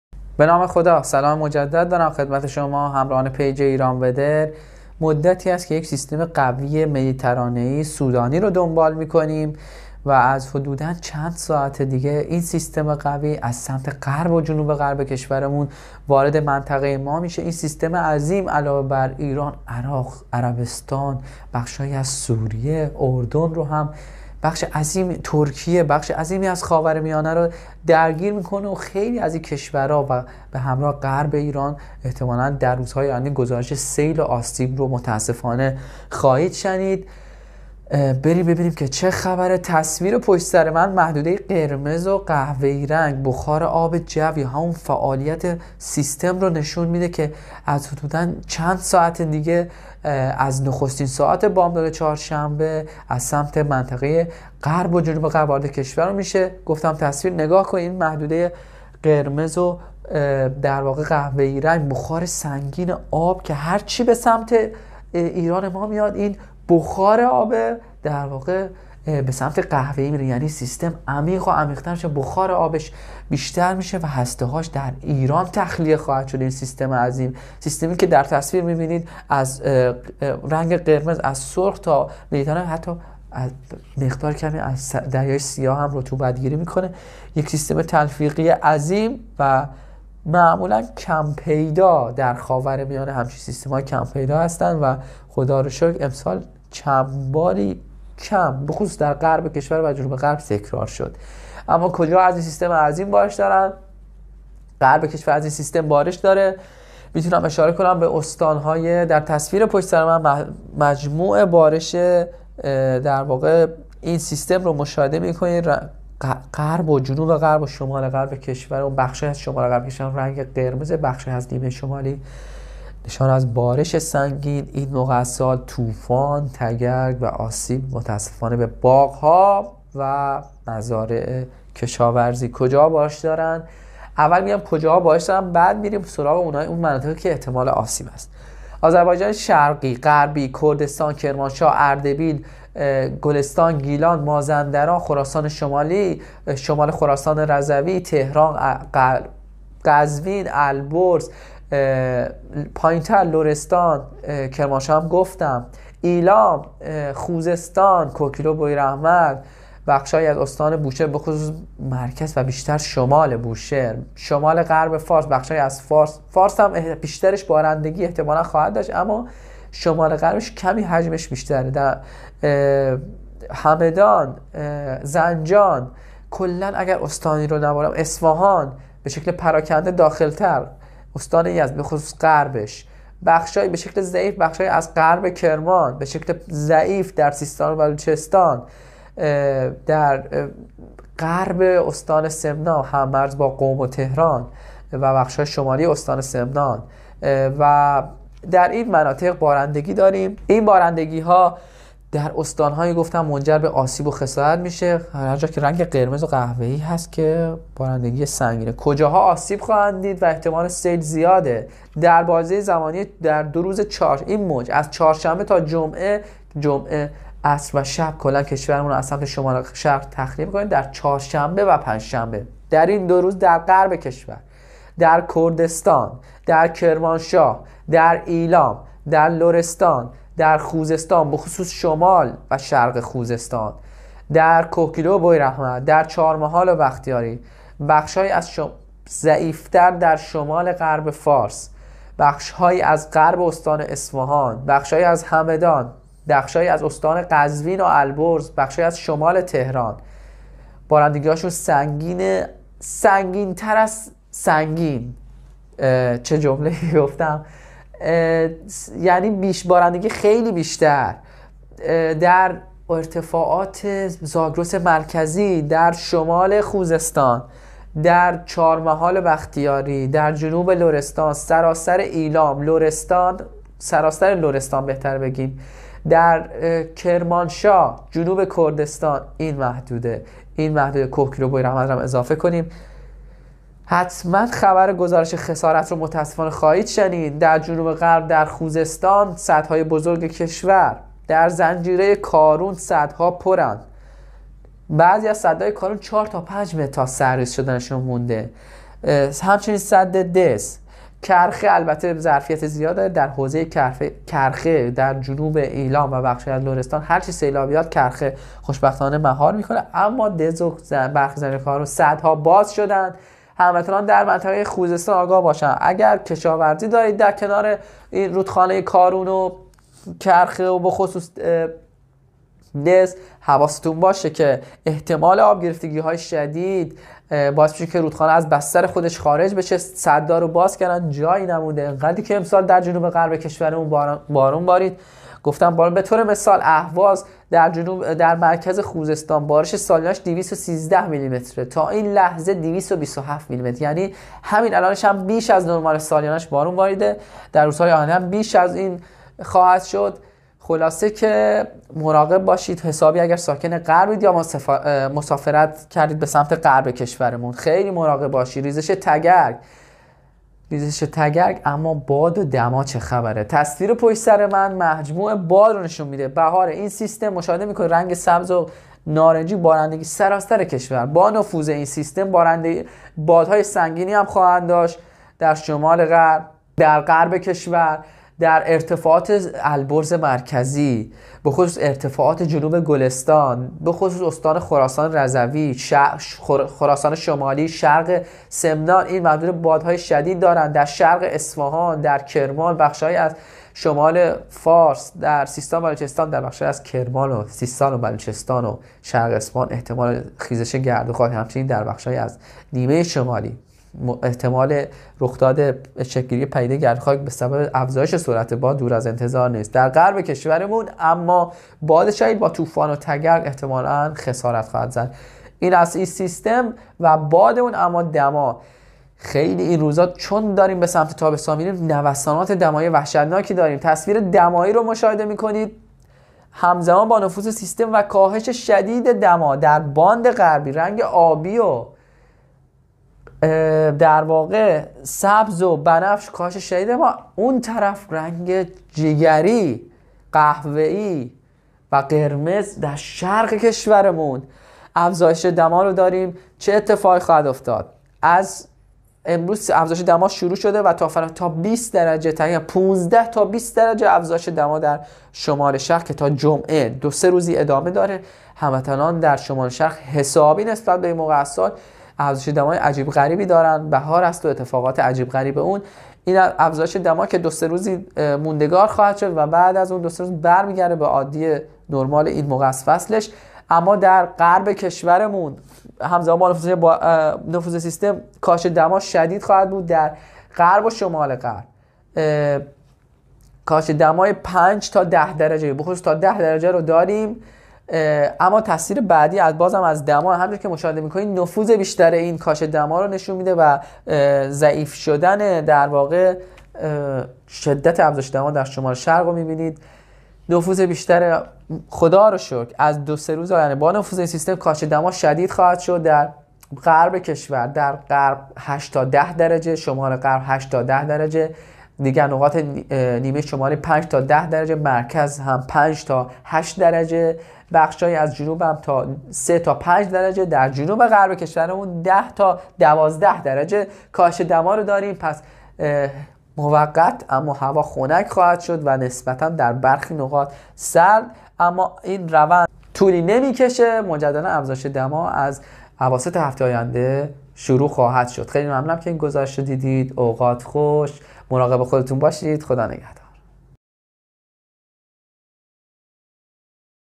گزارش هواشناسی ۲۲ فرودین ۱۴۰۲
ورود یک موج بارشی قوی به غرب کشور

کاهش دمای هوا در اغلب نقاط کشور

هشدار سیل و خسارت ناشی از طوفان